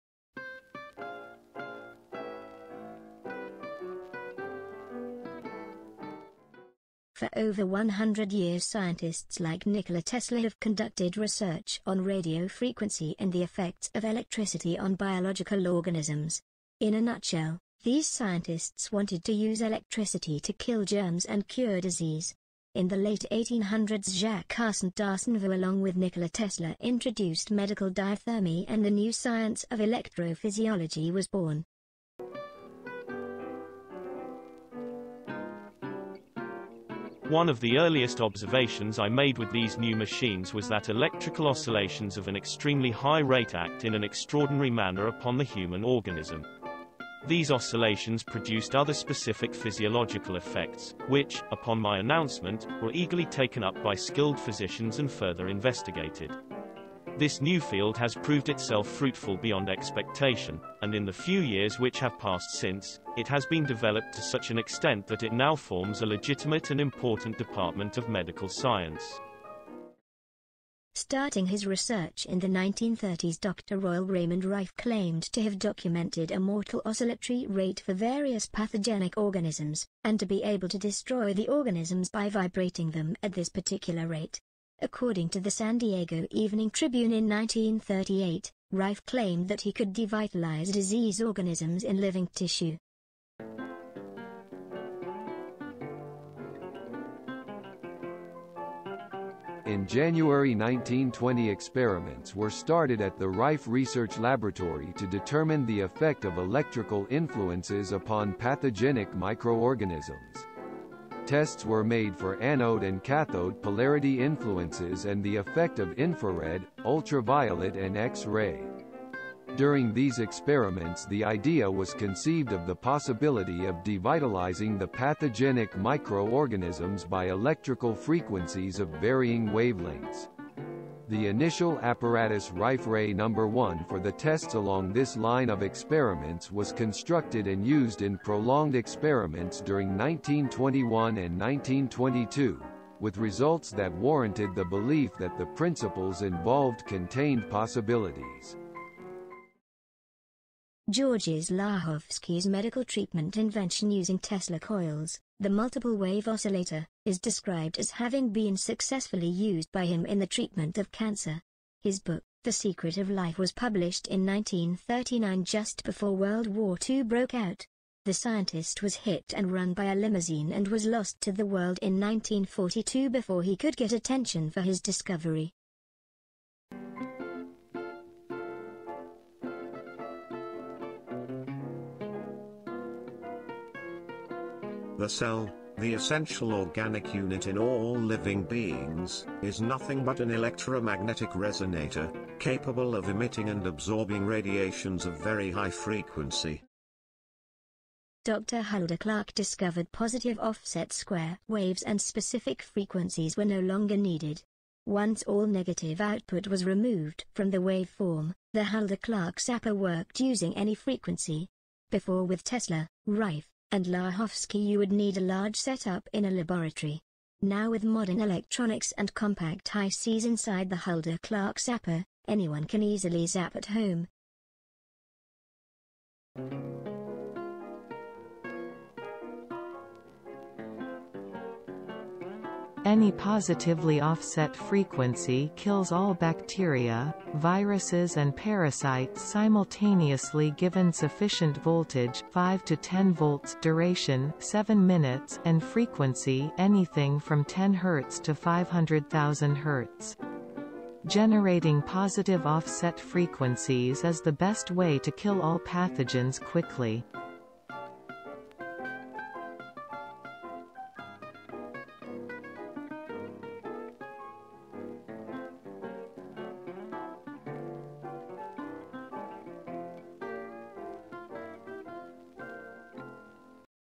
Zapper History
The history of Frequency Therapy began with Nikola Tesla who pioneered the study of radio frequency healing he called medical diathermy. Next came Royal Rife who invented the Rife Ray to destroy microorganisms and Georges Lakhovsky who innovated the Tesla coil into a Multi Wave Oscillator. Hulda Clark simplified the previous large frequency devices using the IC chip to make a portable handheld device to achieve the same results as her predecessors, now called the Clark Zapper.